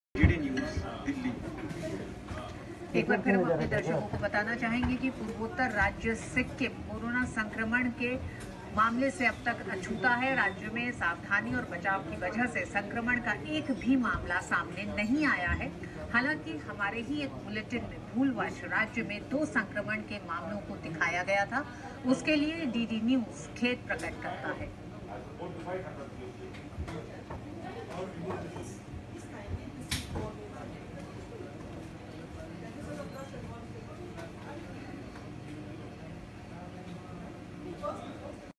DD news#appologize#sikkimese peoples
Reputed national media house DD news  apologize for their report  saying  that  sikkim  has two  COVID19 case.
 but  till  date sikkim has  zero  COVID19 case.